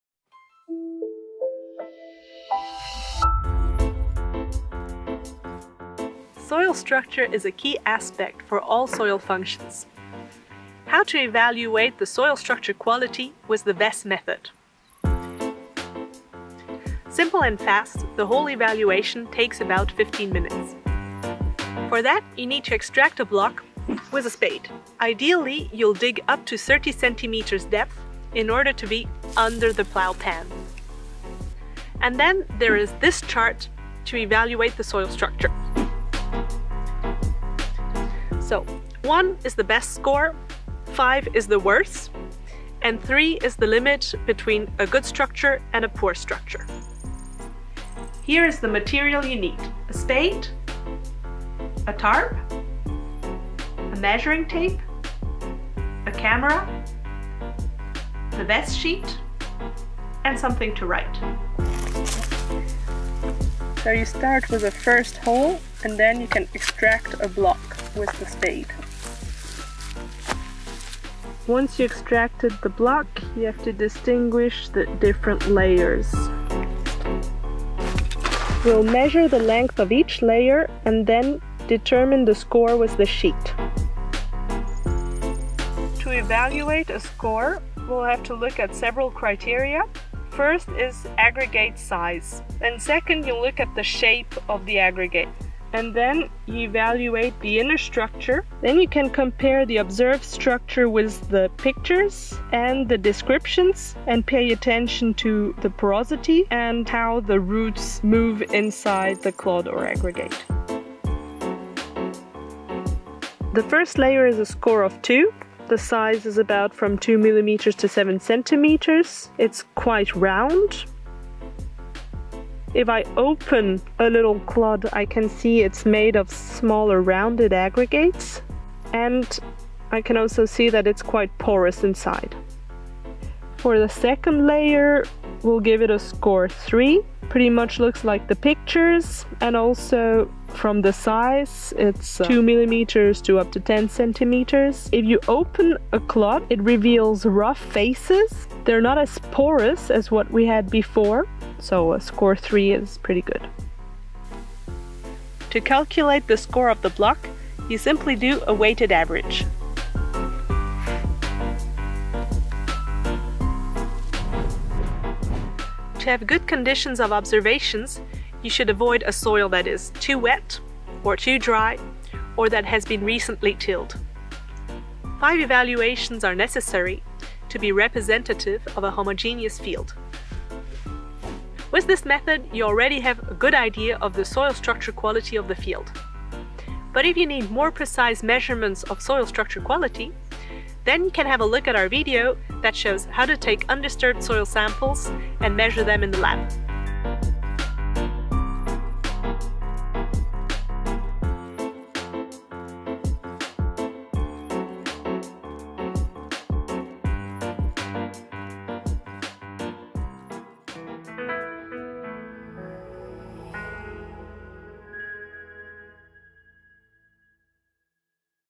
Visual Evaluation of Soil Structure - VESS method
This video explains how to use the VESS method (Guimaraes et al., 2011 ; Ball et al., 2007) to evaluate the soil structure quality.
 (as well as the SpadeSubVESS sheet for the evaluation of subsoils)

The video is presented by Alice Johannes and Cédric Deluz
Scientific expertise: Johannes (Agroscope), Peter Weisskopf (Agroscope) and Pascal Boivin (HES-SO)
The video was filmed and directed by Cédric Deluz

Supported by the Federal Office of Environment (FOEN)n in the frame of the STRUDEL project